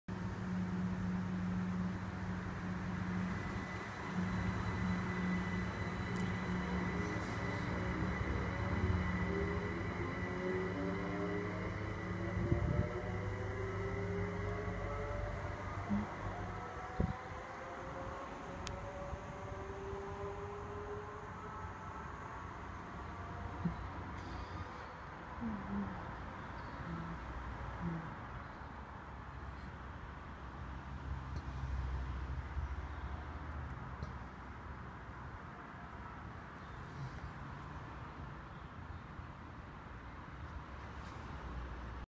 Greater Anglia Class 379 Departing Harlow Mill
Heading Northbound Service to Cambridge North